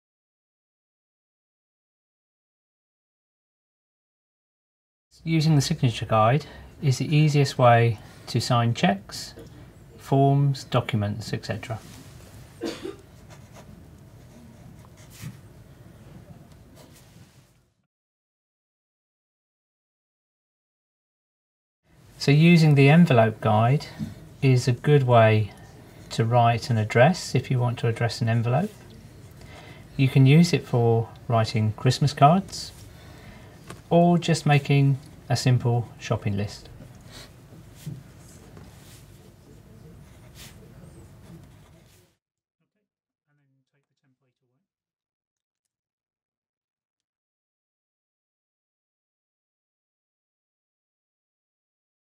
Support 4 Sight - Writing Guides
How writing guides help someone who is visually impaired write addresses and sign documents